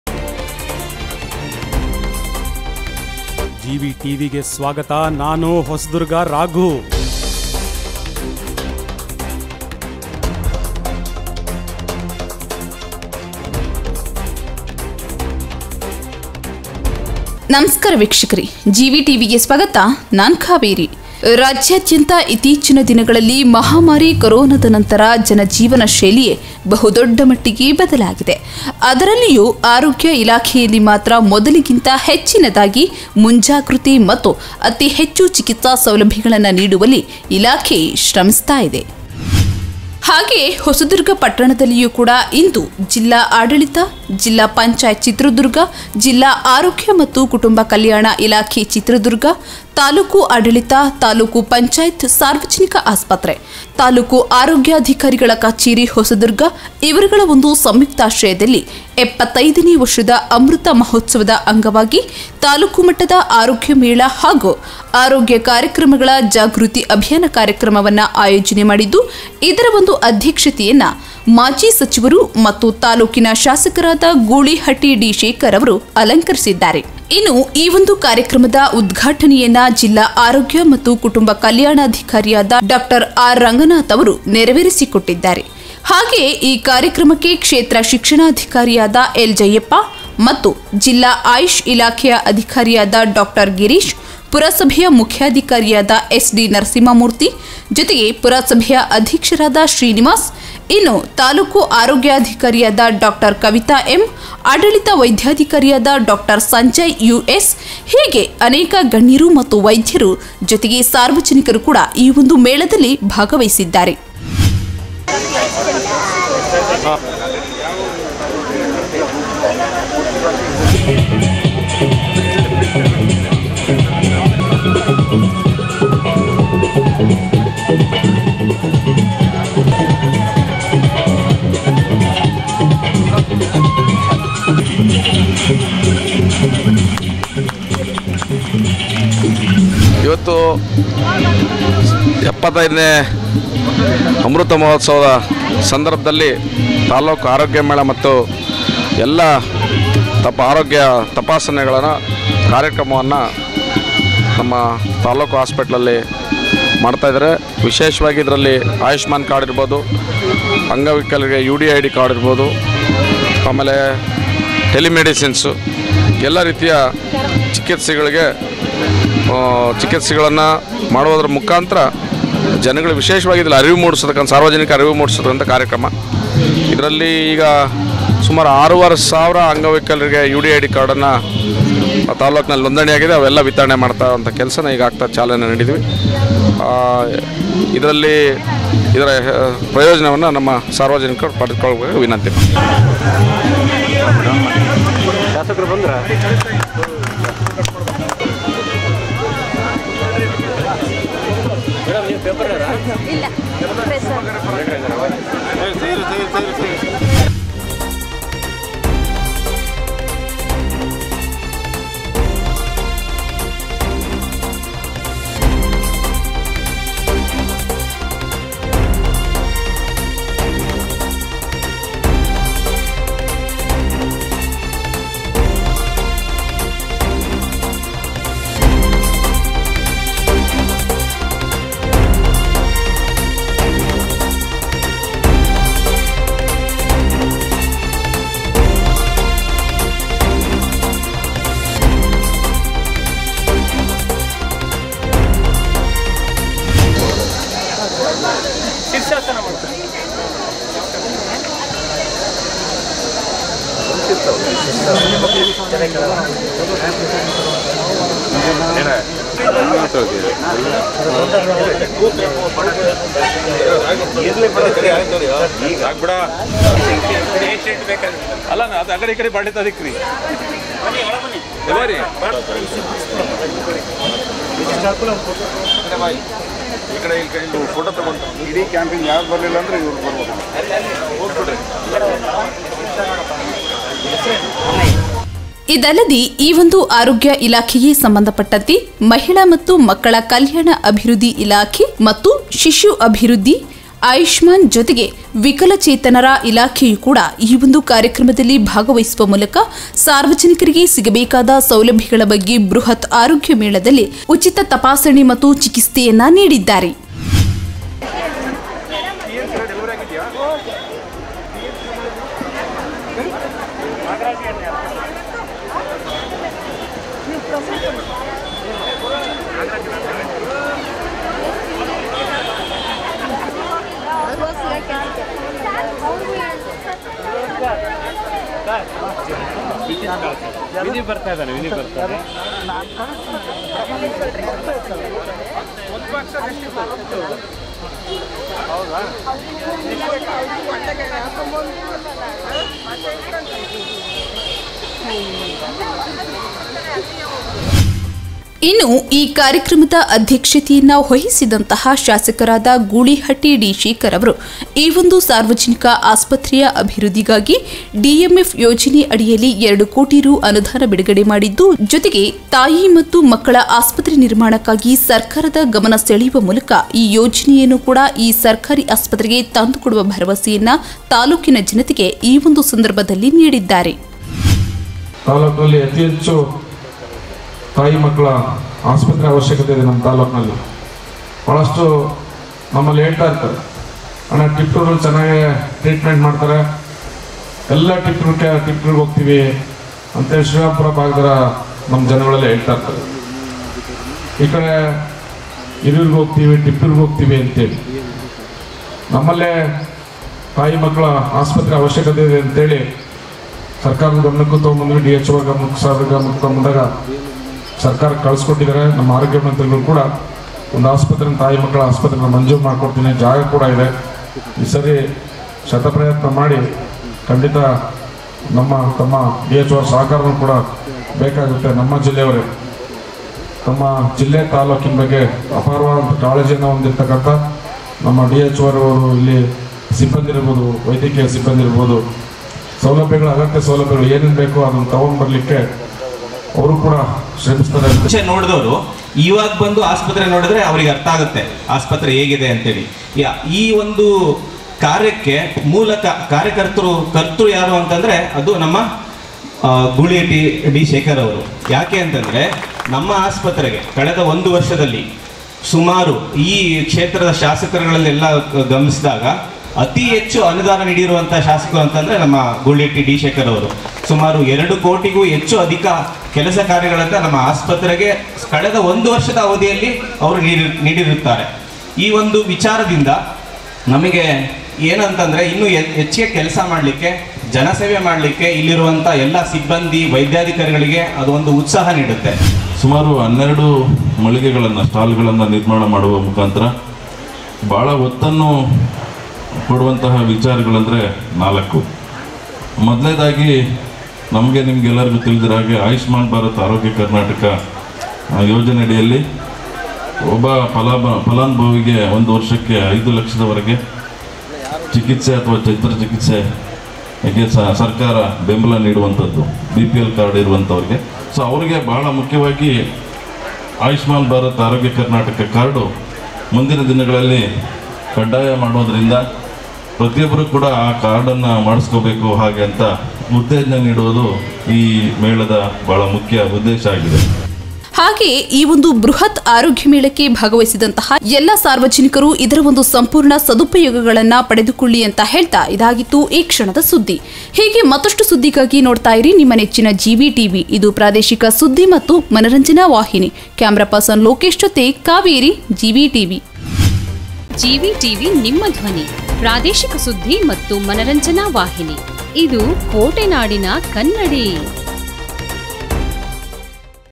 ಹೊಸದುರ್ಗ-ಸಾರ್ವಜನಿಕ ಆಸ್ಪತ್ರೆ ಆವರಣದಲ್ಲಿ ಯಶಸ್ವಿಯಾಗಿ ಜರುಗಿದ ತಾಲ್ಲೂಕು ಮಟ್ಟದ"ಆರೋಗ್ಯ ಮೇಳ".GVTV
75ನೇ ವರ್ಷದ ಅಮೃತ ಮಹೋತ್ಸವದ ಅಂಗವಾಗಿ ಬೃಹತ್ ಆರೋಗ್ಯ ಮೇಳ.